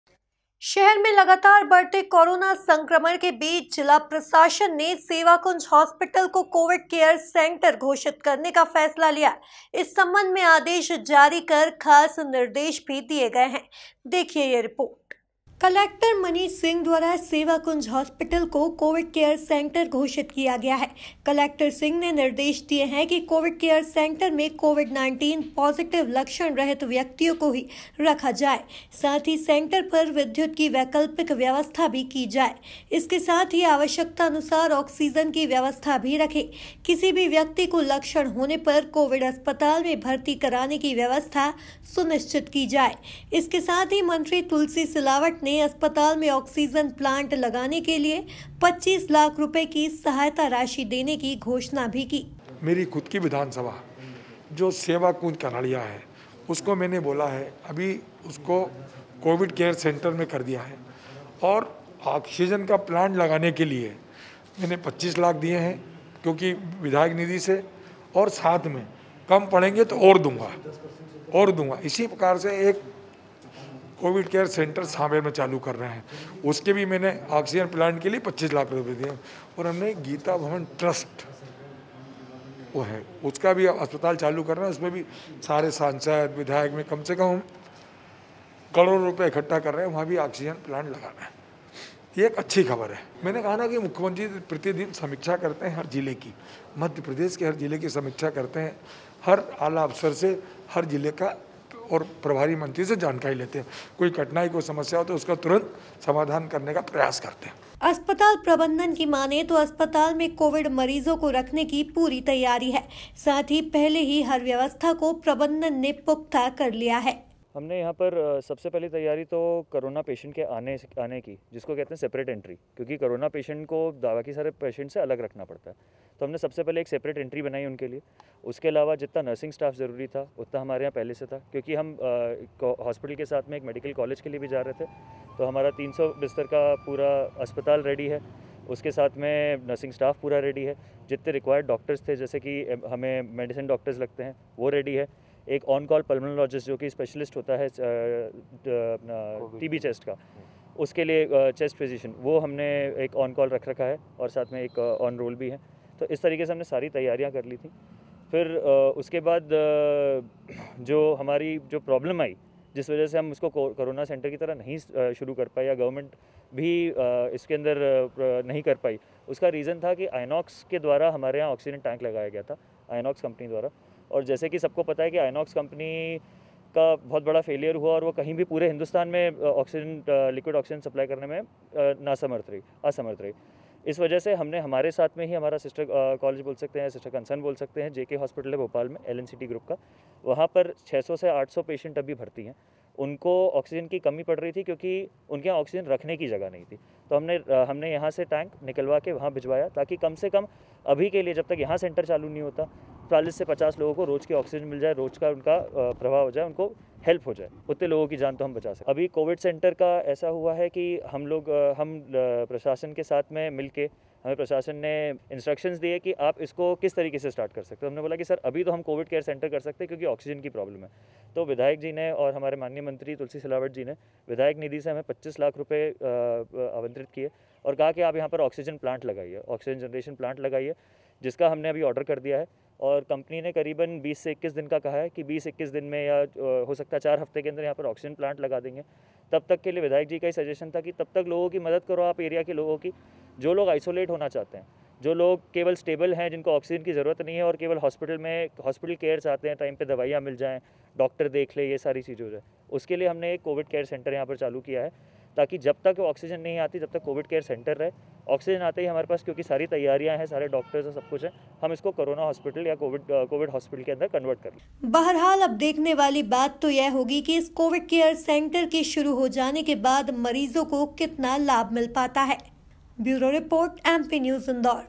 सेवाकुंज अस्पताल बना कोविड सेंटर, ऑक्सीजन प्लांट के लिए मंत्री तुलसी सिलावट ने दिए 25 लाख! MP News
सेवाकुंज अस्पताल बना कोविड सेंटर, ऑक्सीजन प्लांट के लिए मंत्री तुलसी सिलावट ने दिए 25 लाख! MP News Indore